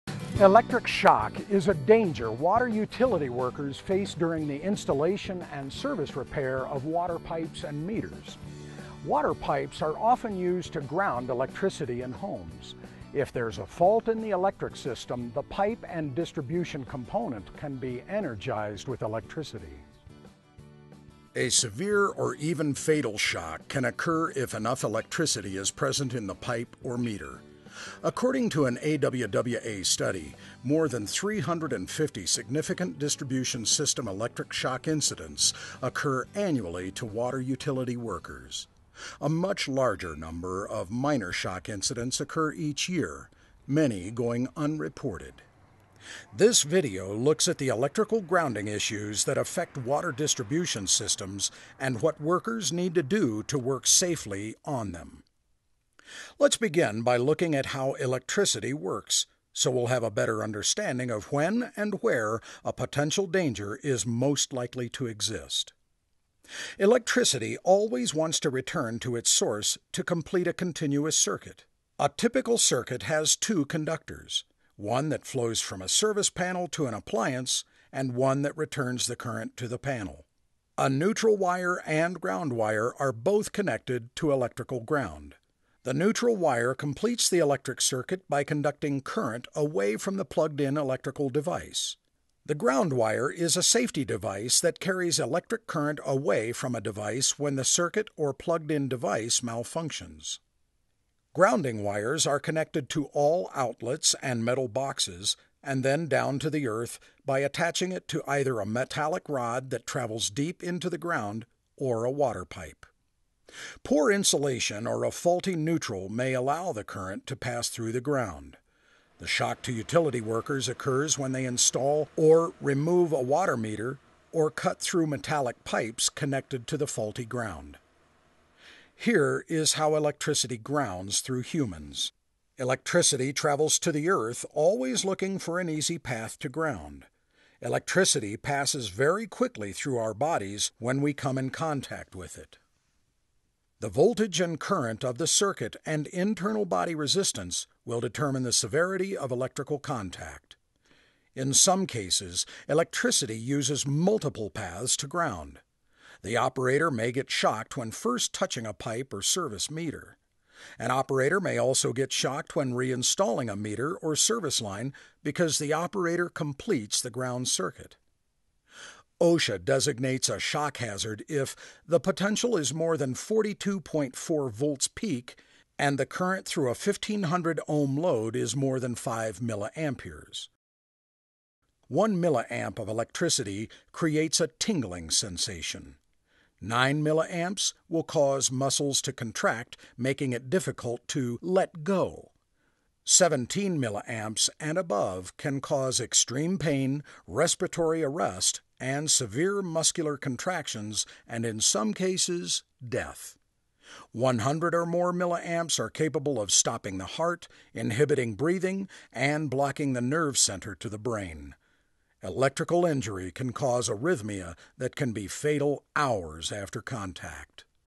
Safety First: Shock Hazards of Electrical Wiring to Water Pipe
Electric shock is a danger water utility workers face during the installation and service repair of water pipes and meters. Water pipes are often used to “ground” electricity in homes. If there is a “fault” in the electric system, the pipe or distribution component can be energized with electricity.